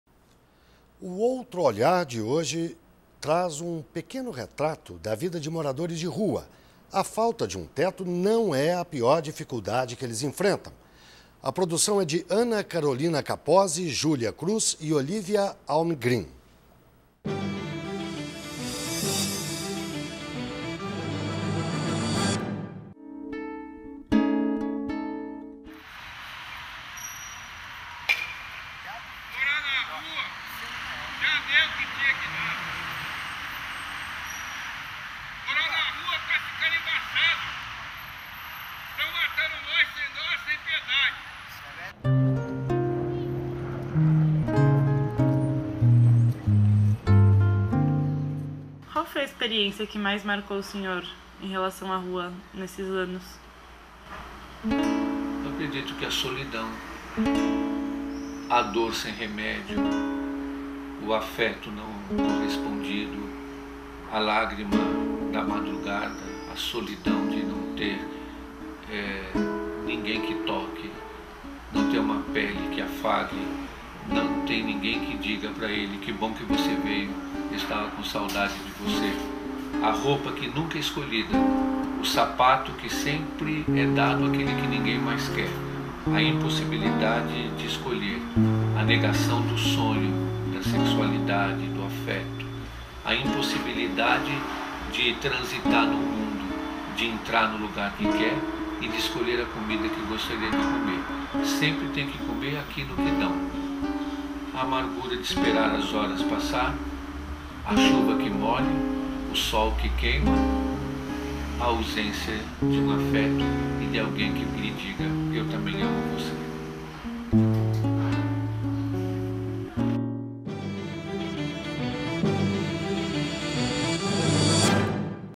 OUTRO OLHAR - Moradores de rua
O Outro Olhar de hoje mostra um pequeno retrato da vida de moradores de rua. A falta de um teto não é a pior dificuldade que eles enfrentam. A produção é de Ana Carolina Capozzi, Júlia Cruz e Olívia Almgrem.